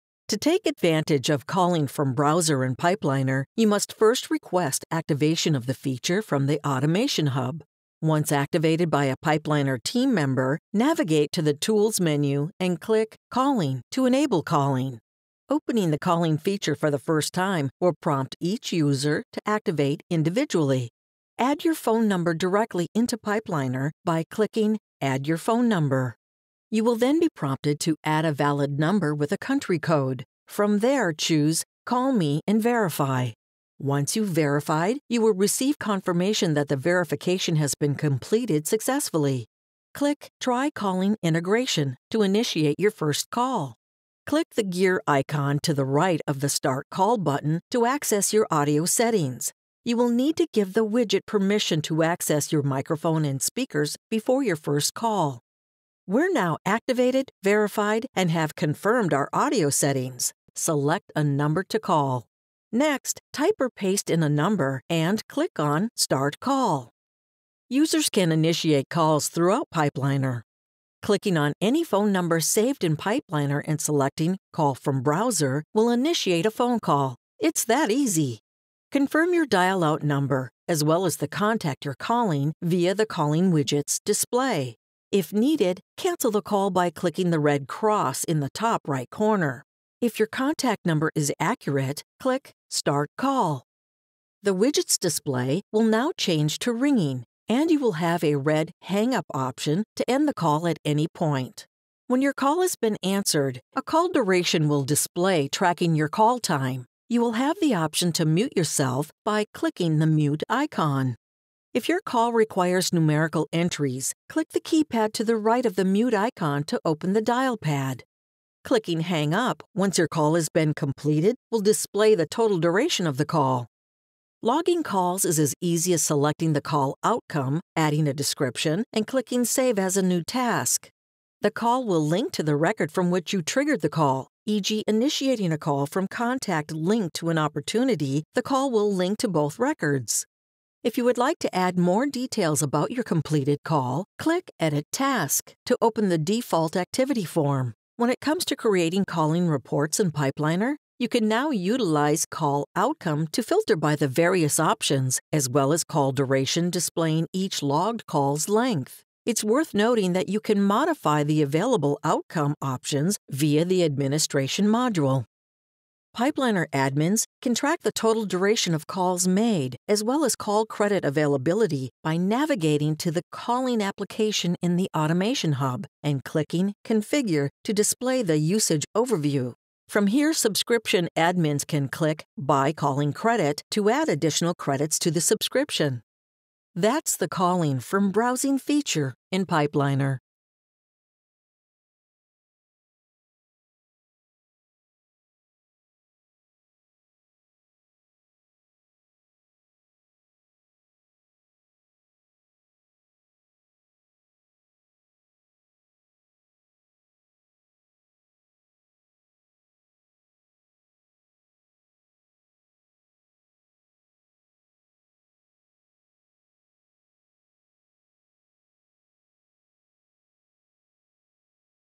Calling from Browser
Use the Pipeliner Calling feature to initiate a call directly from your Pipeliner cloud web application and then log the call as a completed task making keeping in touch with your customers and prospects even more seamless and efficient.